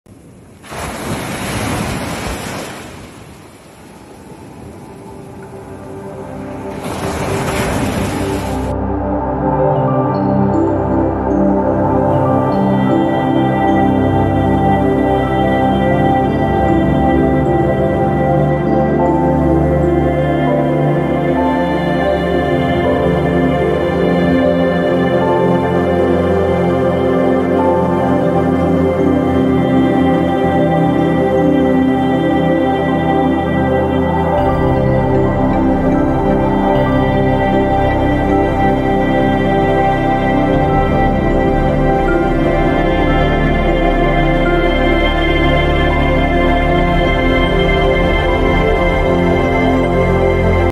Phuket Traveller Reviews - Baan Laimai - Patong Beach Resort - Short Video
Experiencing beauty & recreation on Patong Beach or alternatively strolling for lively entertaining & shopping.

 5 steps to Patong Beach
 5 mins walk to Bangla Road
 10 mins walk to Jungceylon
 30 mins drive to the International Hospitals
 60 mins drive to Phuket International Airport